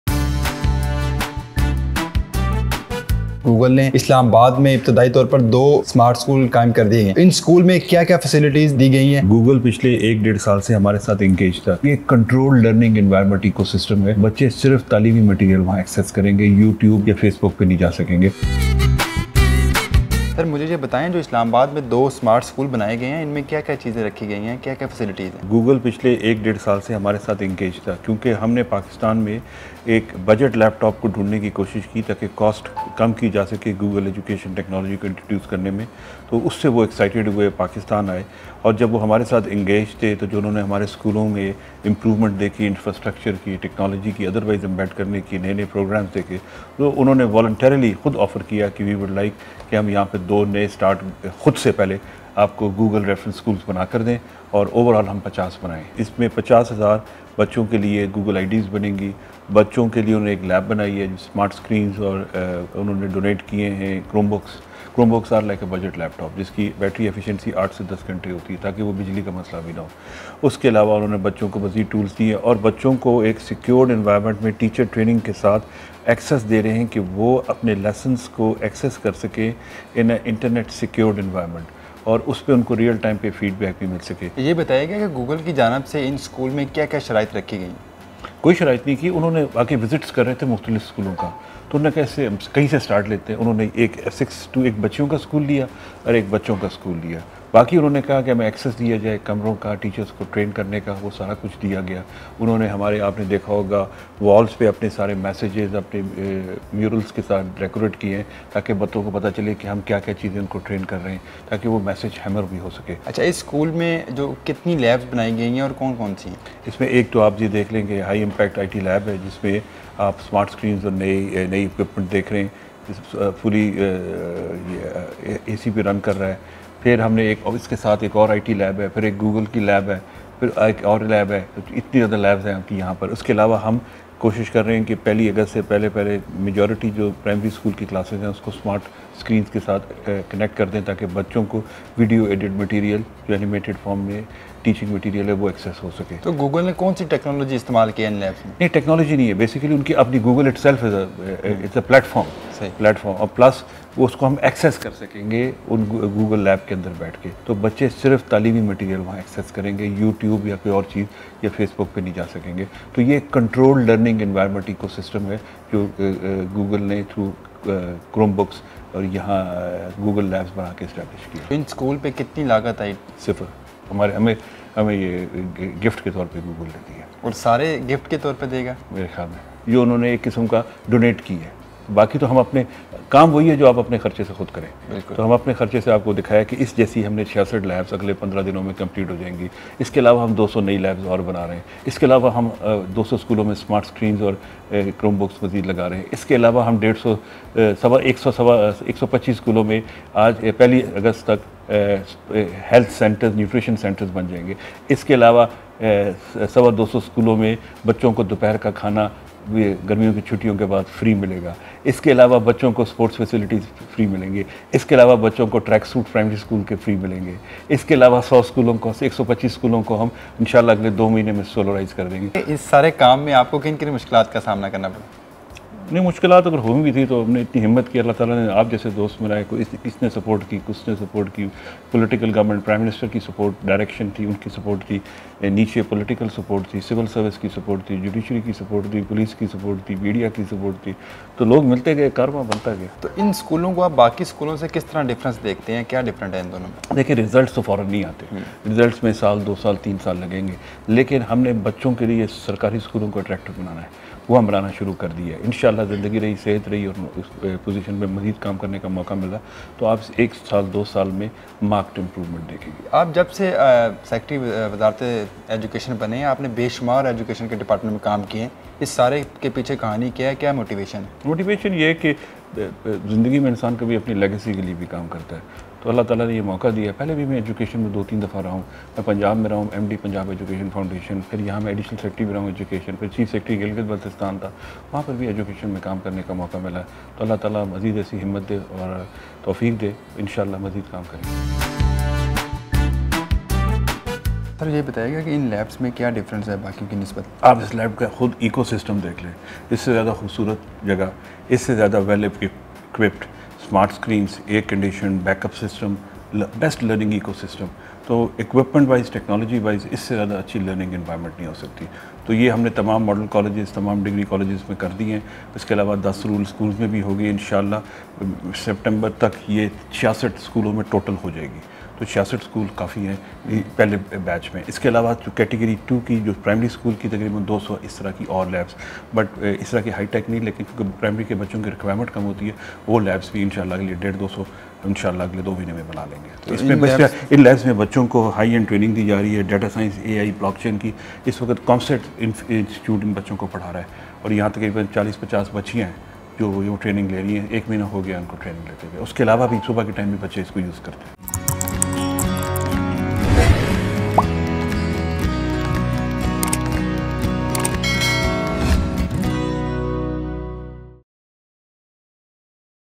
Google Built 2 Beautiful Smart Schools In Islamabad
Discover the stunning smart schools built by Google in Islamabad, showcasing innovative design and cutting-edge technology to enhance the learning experience for students. Explore the modern facilities and sustainable features that make these schools truly unique.
Anchor: Ameer Hamza